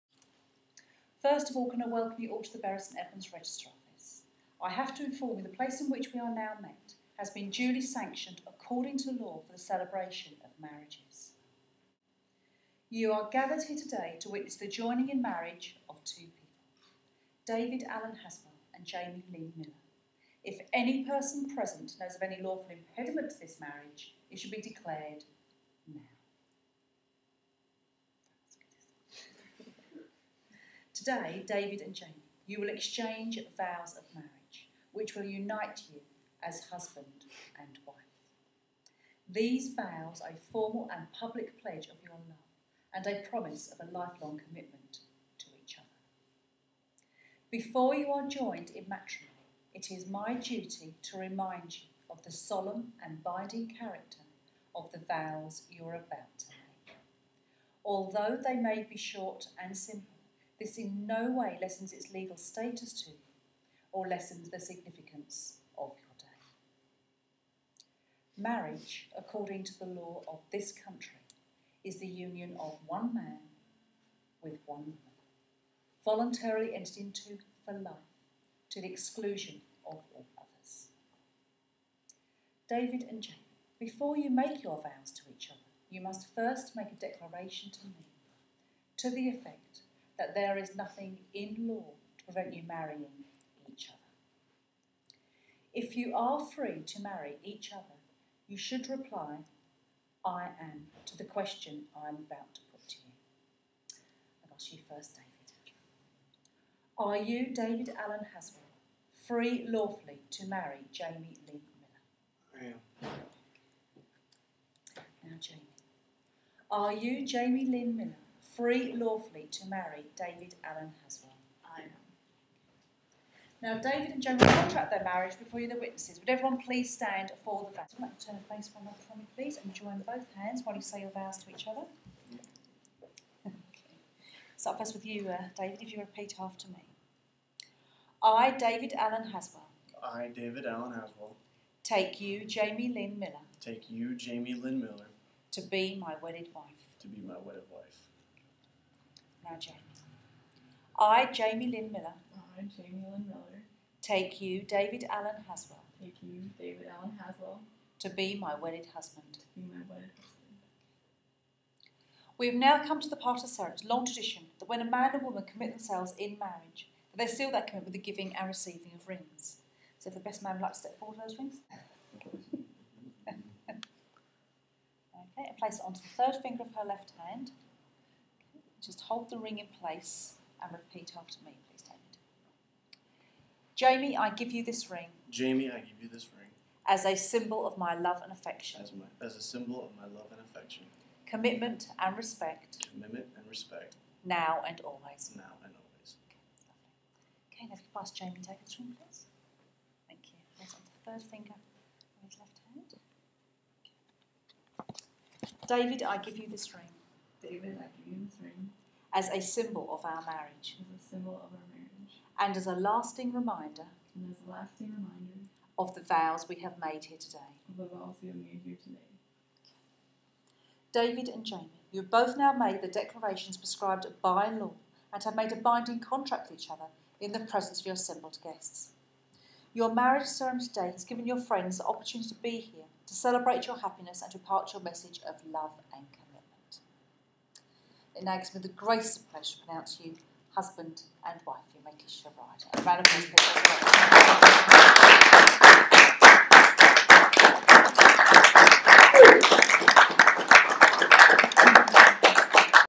Mr & Mrs Haswell
Our wedding for our family and friends who couldn't make it! We love you and wanted to make sure we shared this absolutely wonderful day with you.